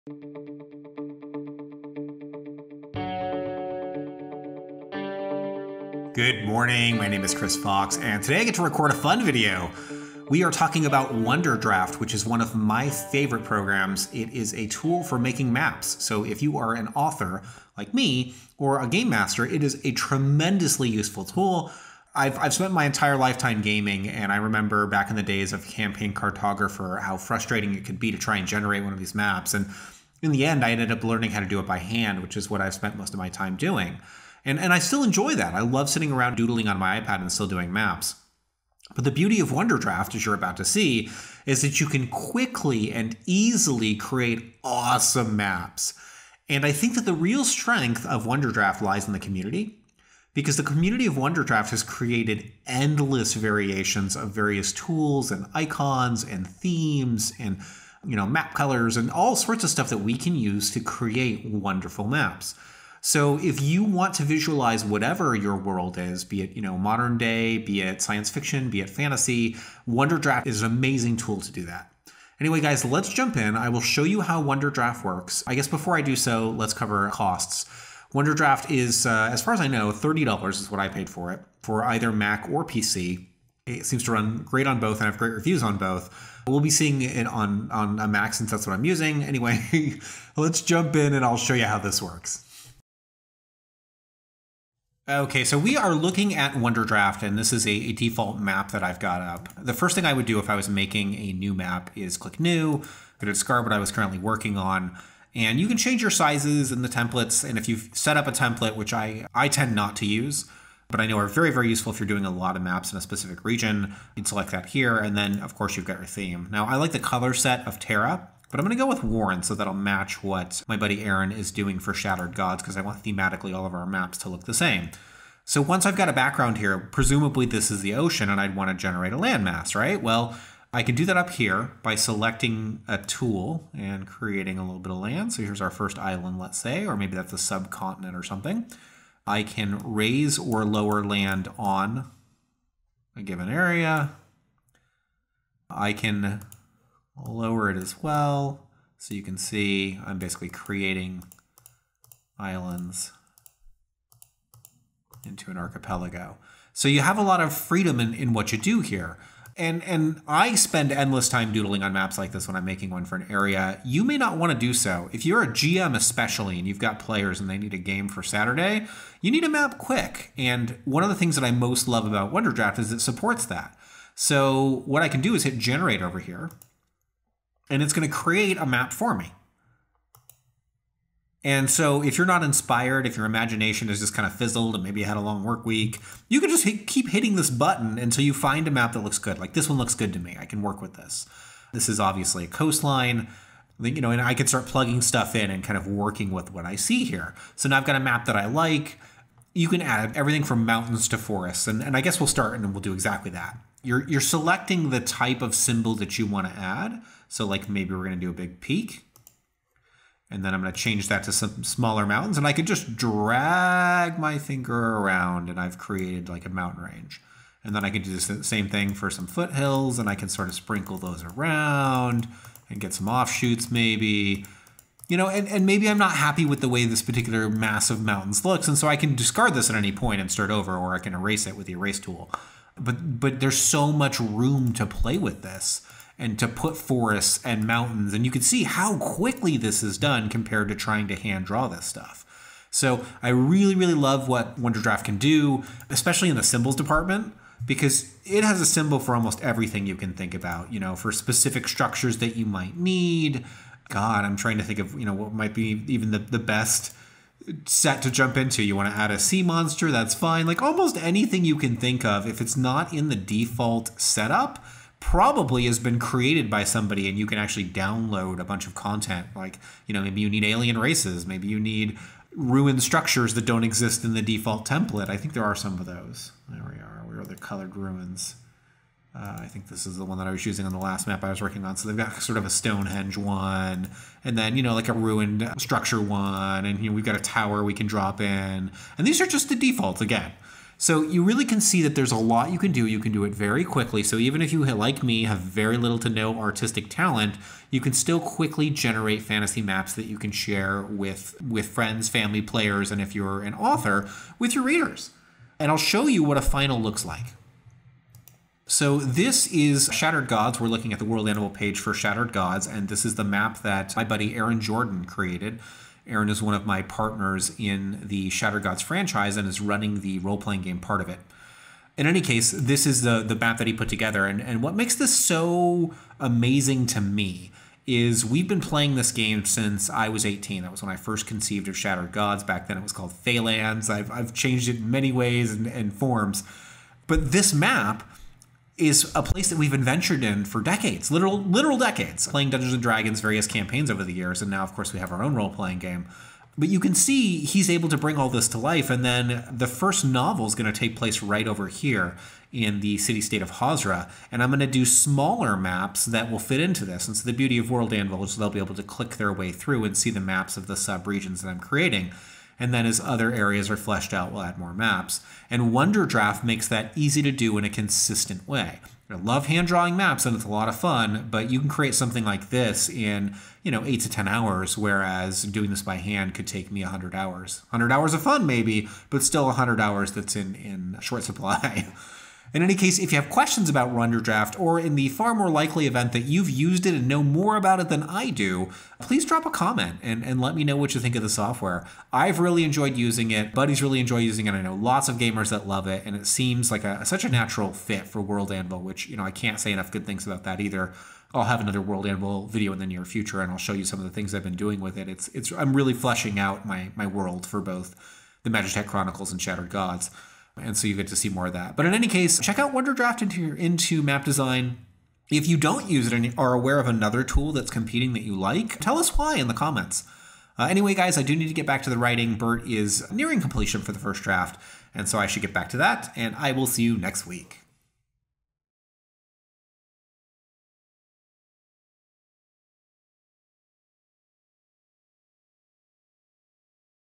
Why Authors Love Wonderdraft for Mapmaking
I HAVE NO FINANCIAL INTEREST IN WONDERDRAFT. I just think it's cool software =)